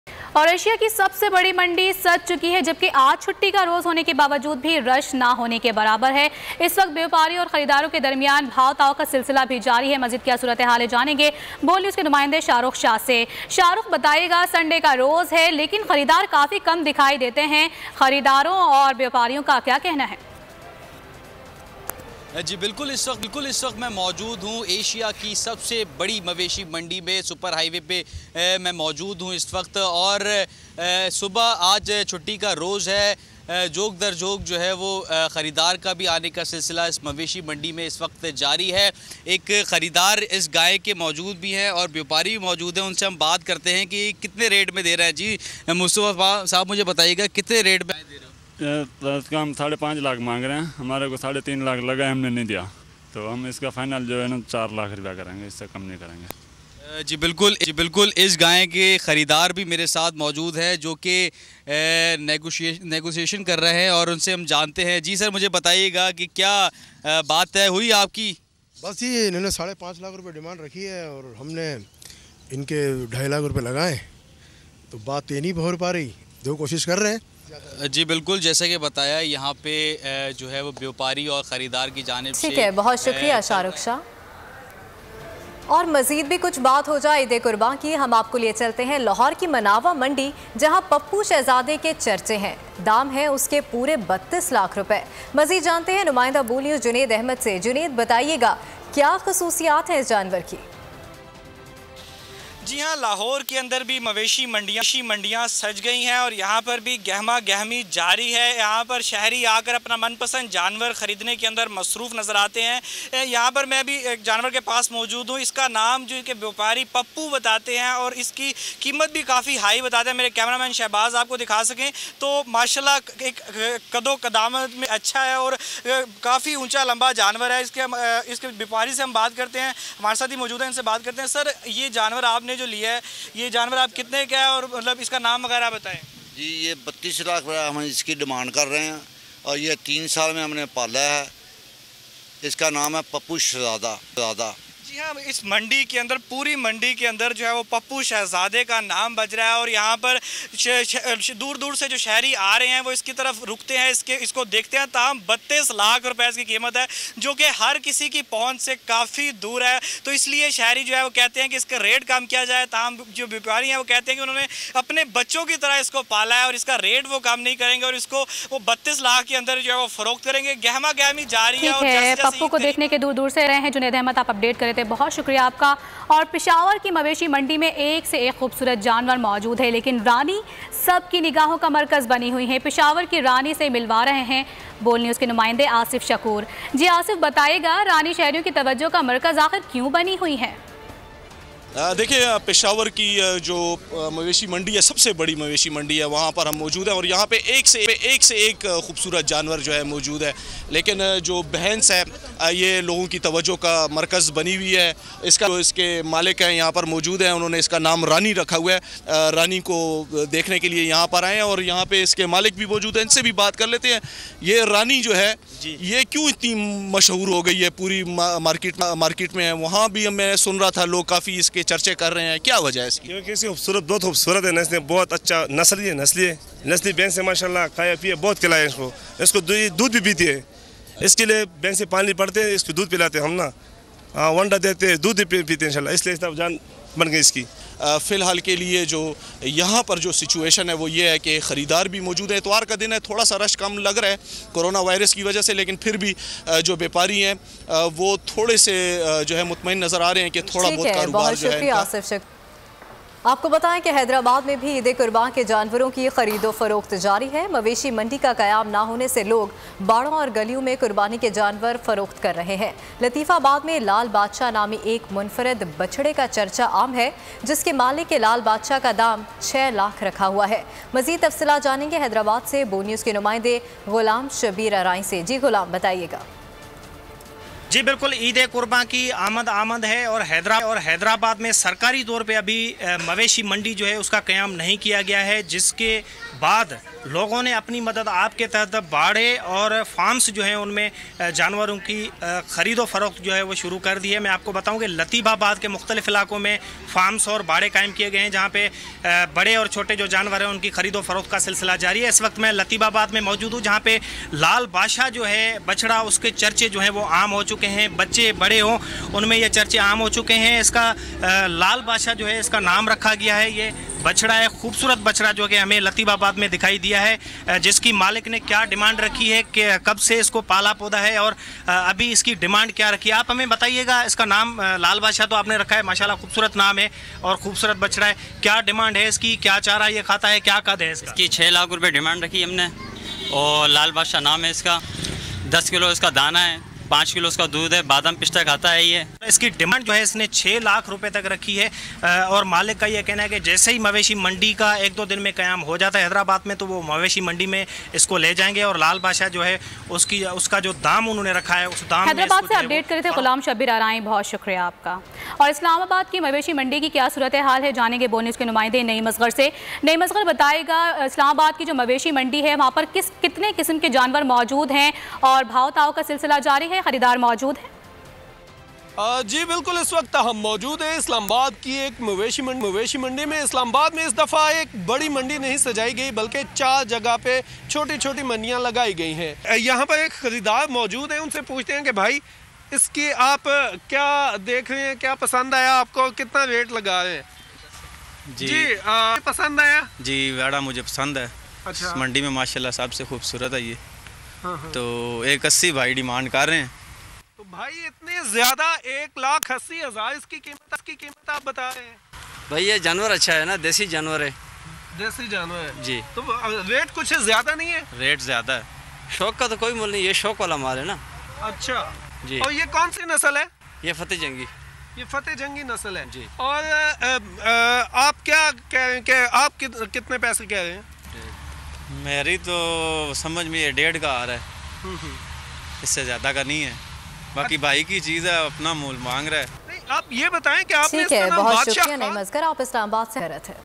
Cattle Market Setup Nationwide | Eid ul Azha 2020 Preparations on Full Swing | BOL News
Cattle Market Setup Nationwide
Eid ul Azha 2020 Preparations on Full Swing